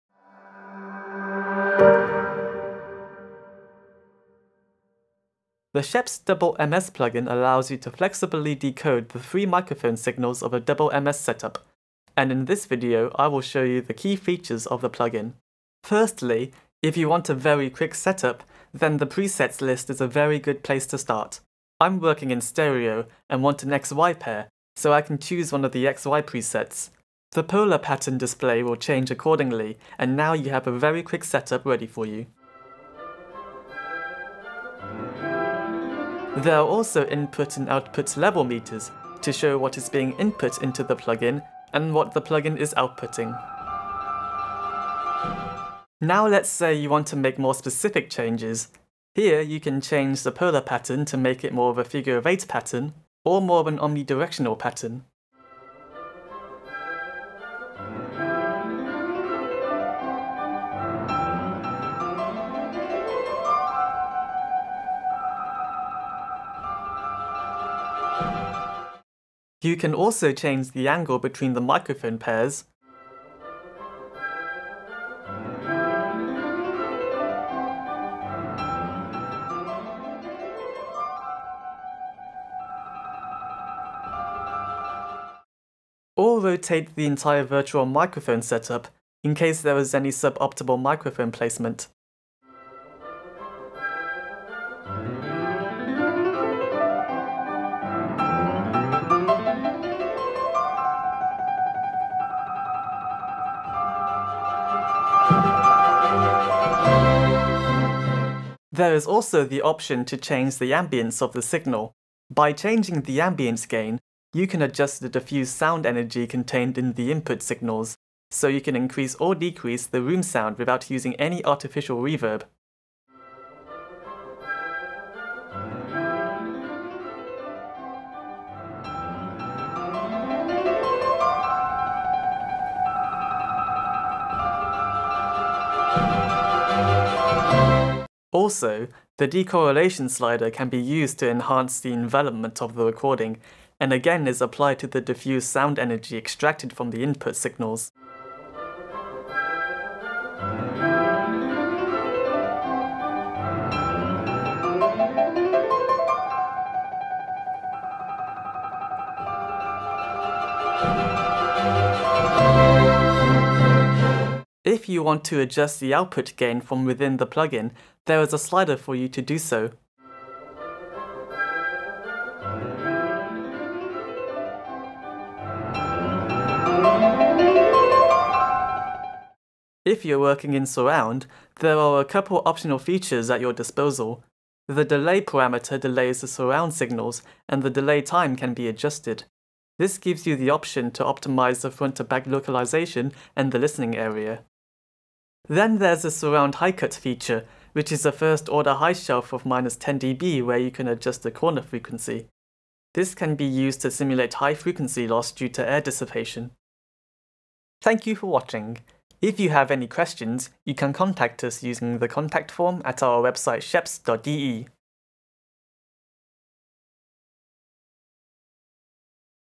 Double MS Plugin Demo
The new, free SCHOEPS Double MS plugin allows tailor-made, intuitive dematrixing of the three signals of the Double MS system.